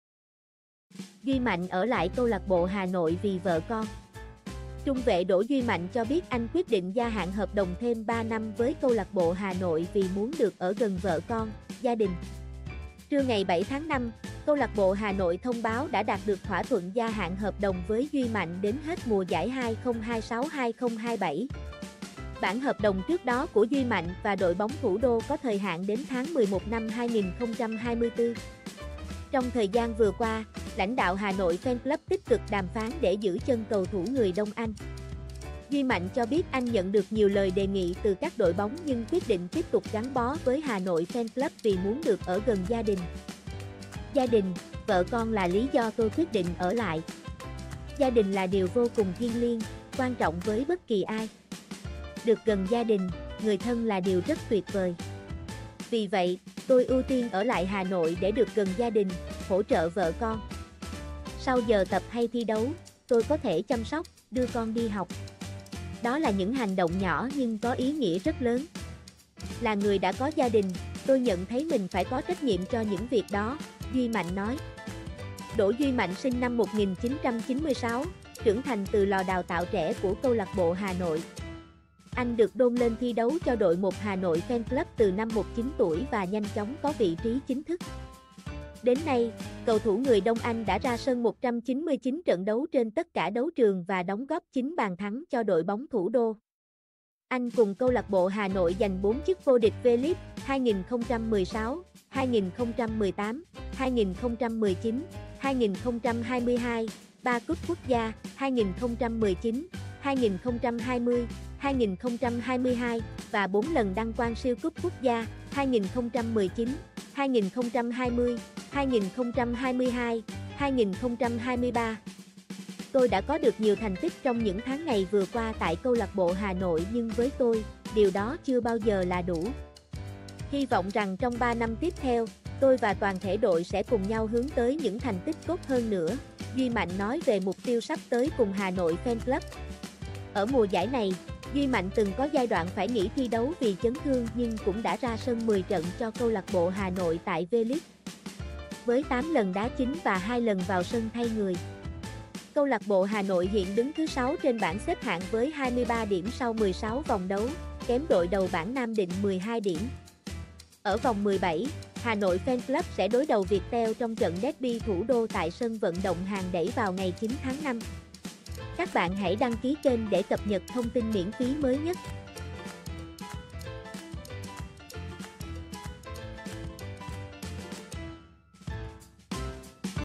Duy Mạnh ở lại CLB Hà Nội vì vợ con
Trung vệ Đỗ Duy Mạnh cho biết anh quyết định gia hạn hợp đồng thêm ba năm với CLB Hà Nội vì muốn được ở gần vợ con, gia đình.

Trưa 7/5, CLB Hà Nội thông báo đã đạt được thỏa thuận gia hạn hợp đồng với Duy Mạnh đến hết mùa giải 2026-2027. Bản hợp đồng trước đó của Duy Mạnh và đội bóng thủ đô có thời hạn đến tháng 11/2024. Trong thời gian vừa qua, lãnh đạo Hà Nội FC tích cực đàm phán để giữ chân cầu thủ người Đông Anh. Duy Mạnh cho biết anh nhận được nhiều lời đề nghị từ các đội bóng nhưng quyết định tiếp tục gắn bó với Hà Nội FC vì muốn được ở gần gia đình.

"Gia đình, vợ con là lý do tôi quyết định ở lại. Gia đình là điều vô cùng thiêng liêng, quan trọng với bất kỳ ai. Được gần gia đình, người thân là điều rất tuyệt vời. Vì vậy, tôi ưu tiên ở lại Hà Nội để được gần gia đình, hỗ trợ vợ con. Sau giờ tập hay thi đấu, tôi có thể chăm sóc, đưa con đi học. Đó là những hành động nhỏ nhưng có ý nghĩa rất lớn. Là người đã có gia đình, tôi nhận thấy mình phải có trách nhiệm cho những việc đó", Duy Mạnh nói.

Đỗ Duy Mạnh sinh năm 1996, trưởng thành từ lò đào tạo trẻ của CLB Hà Nội. Anh được đôn lên thi đấu cho đội một Hà Nội FC từ năm 19 tuổi và nhanh chóng có vị trí chính thức. Đến nay, cầu thủ người Đông Anh đã ra sân 199 trận đấu trên tất cả đấu trường và đóng góp 9 bàn thắng cho đội bóng thủ đô. Anh cùng CLB Hà Nội giành 4 chức vô địch V-League (2016, 2018, 2019, 2022), ba Cup Quốc gia (2019, 2020, 2022) và 4 lần đăng quang Siêu Cup Quốc gia (2019, 2020, 2022, 2023).

"Tôi đã có được nhiều thành tích trong những tháng ngày vừa qua tại CLB Hà Nội nhưng với tôi, điều đó chưa bao giờ là đủ. Hy vọng rằng trong ba năm tiếp theo, tôi và toàn thể đội sẽ cùng nhau hướng tới những thành tích tốt hơn nữa", Duy Mạnh nói về mục tiêu sắp tới cùng Hà Nội FC.

Ở mùa giải này, Duy Mạnh từng có giai đoạn phải nghỉ thi đấu vì chấn thương nhưng cũng đã ra sân 10 trận cho CLB Hà Nội tại V-League, với 8 lần đá chính và hai lần vào sân thay người. CLB Hà Nội hiện đứng thứ 6 trên bảng xếp hạng với 23 điểm sau 16 vòng đấu, kém đội đầu bảng Nam Định 12 điểm. Ở vòng 17, Hà Nội FC sẽ đối đầu Viettel trong trận derby thủ đô tại sân vận động Hàng Đẫy vào ngày 9/5.